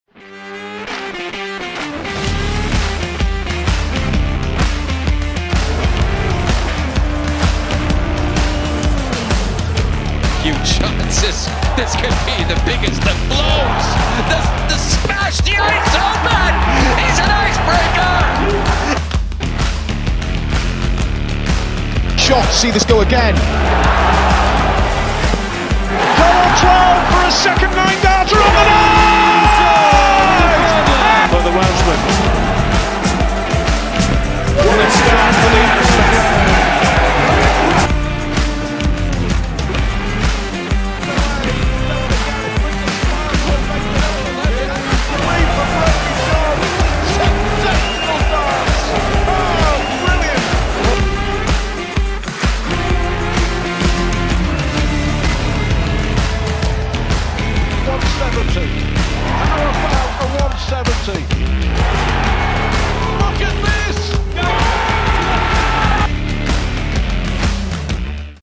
Wigan 2024 - 19th September - A Night at the Darts
MDA and MODUS will be in Wigan's DW stadium for a massive night of Darts